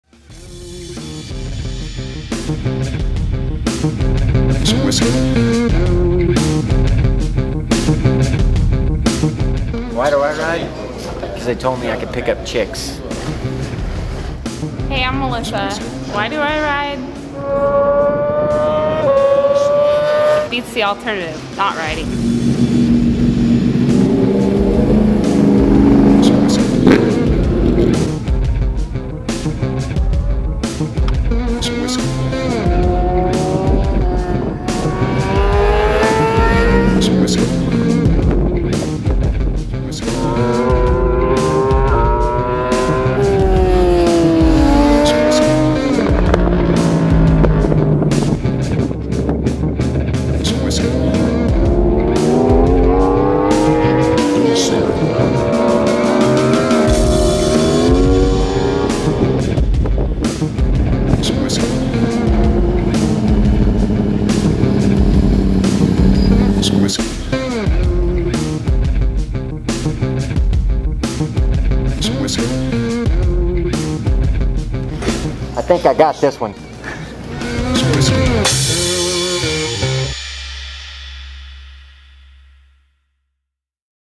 Josh Hayes Tire Testing in Las Vegas Explains Pick Up - 101
Team Monster Energy - Graves Motorsports - Yamaha AMA Pro Racing Superbike Champion Josh Hayes explains how he uses his Yamaha R1 to pick up girls. Why Do You Ride? - YWeRide.com